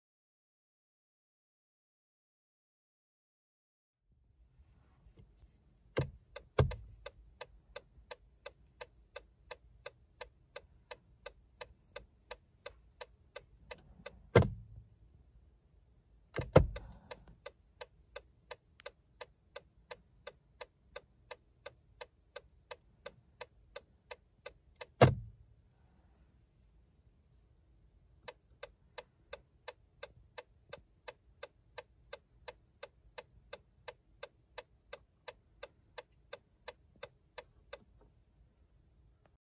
2020 Honda Insight Hybrid EX | Turn Signal Sounds
Here’s a requested video of the turn signal sound on my 2020 Honda Insight Hybrid EX.